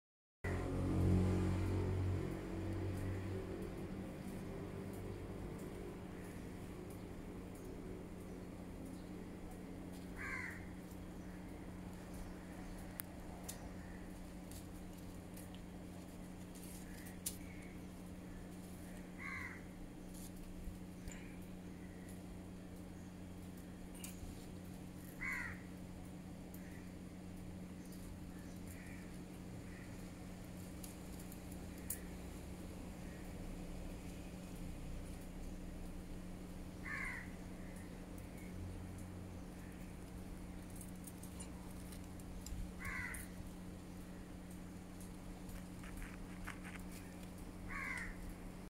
Dwarf Honey Bees, Apis Florea Pt 2
Beekeeping in my backyard.